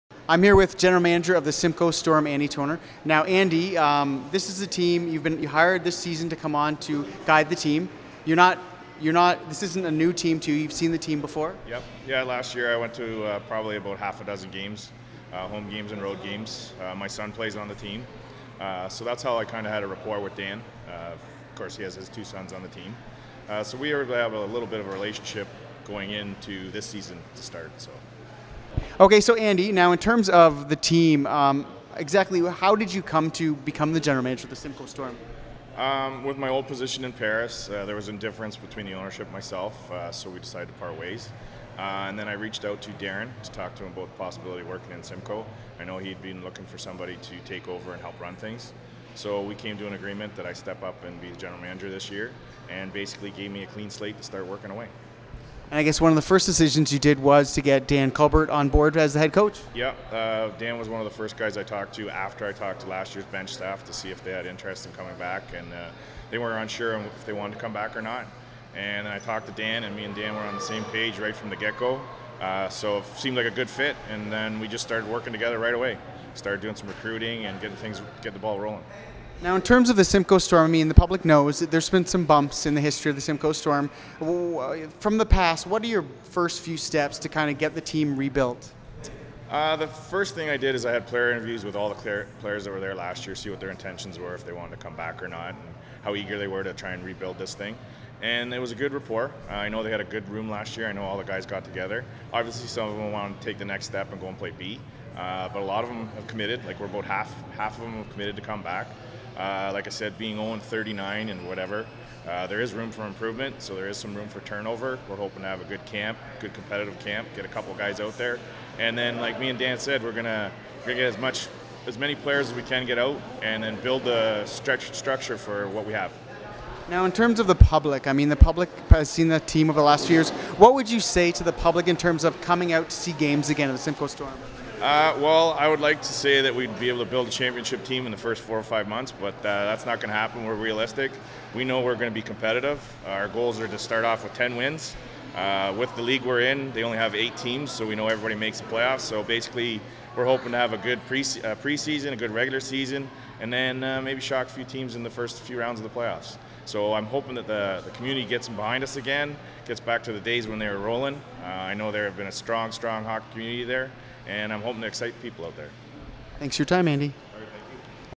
Andy Tonner Interview
Interview with Simcoe Storm General Manager Andy Tonner.